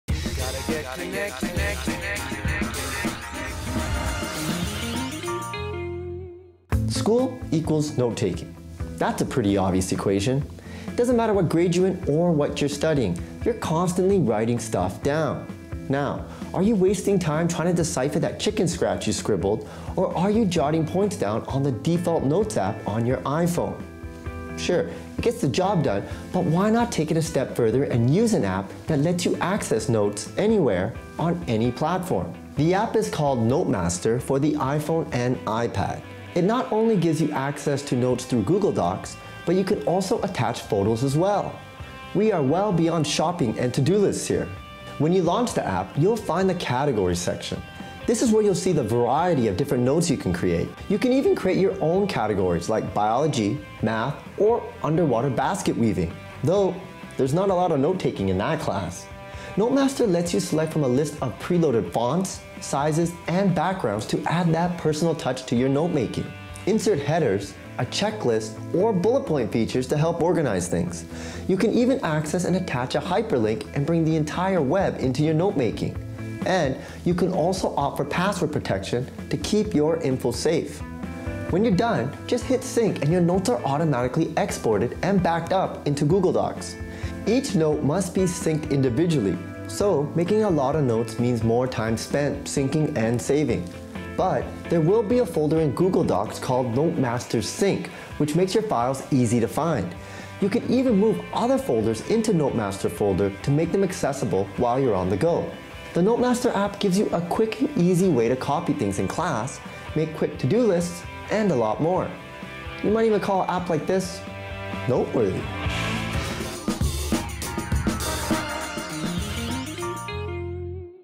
Students Can Improve Note Taking on the iPhone with NoteMaster
Note taking is an important skill for students. We look at a way to improve note taking on the iPhone with a new app called NoteMaster. It provides additional features not available in the default iPhone notes app like syncing with Google Docs, image embeds and even linking websites from your notes.